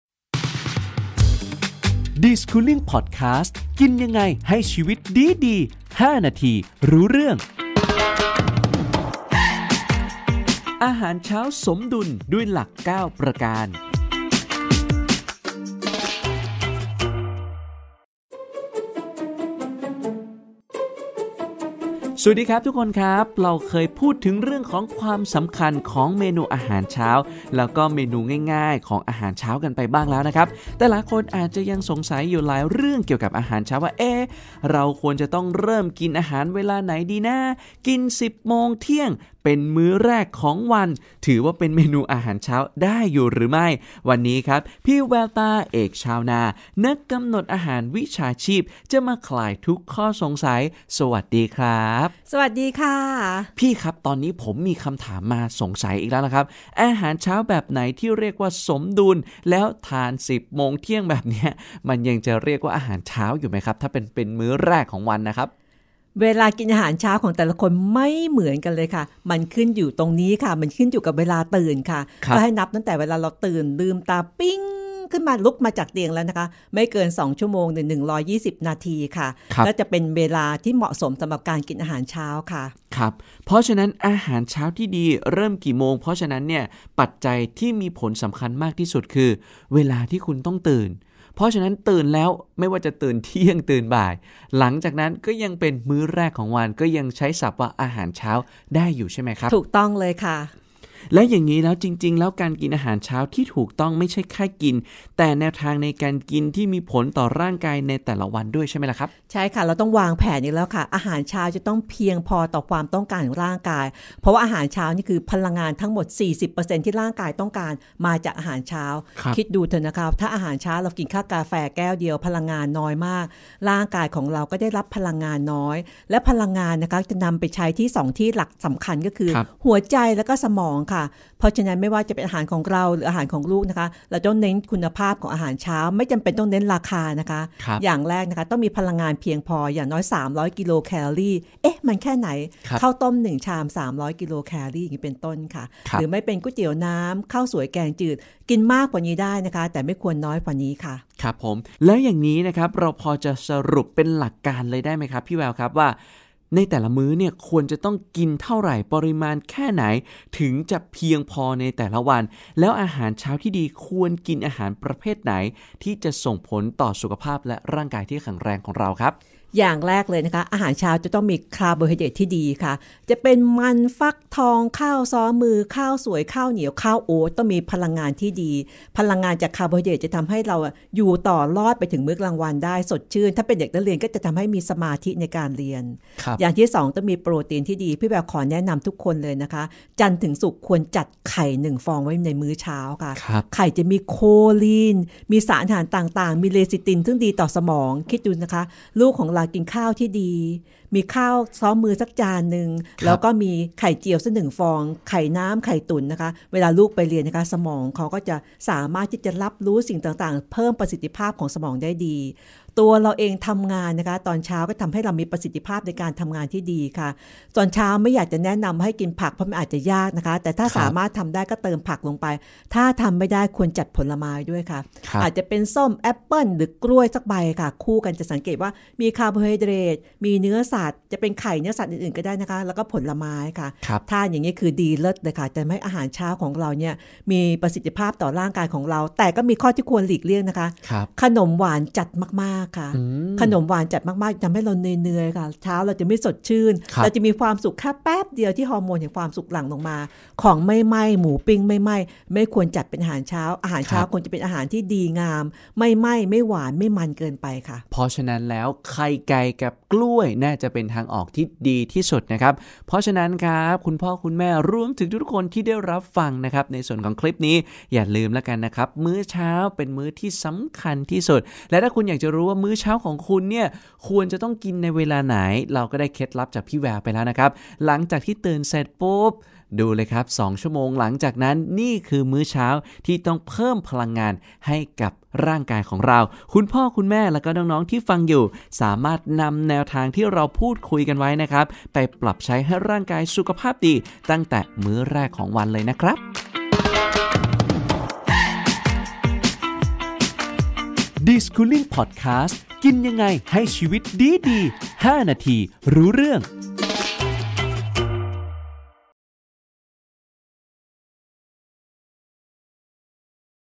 อาหารเช้าสมดุล เริ่มที่คุณได้ทันที | Deschooling Podcast
อาหารเช้า สำคัญต่อการดำเนินชีวติของคนเรามาก ๆ แต่บางครั้งวิถีชีวิตที่เร่งรีบก็ทำให้หลายคนไม่สามารถรับประทานอาหารเช้าได้ แต่เลือกที่จะรับประทานอาหารในช่วงเวลาที่ตนเองสะดวก พฤติกรรมแบบนี้จะสามารถทดแทนกันได้หรือไม่ และอาหารเช้าแบบไหนถึงจะดีต่อร่างกาย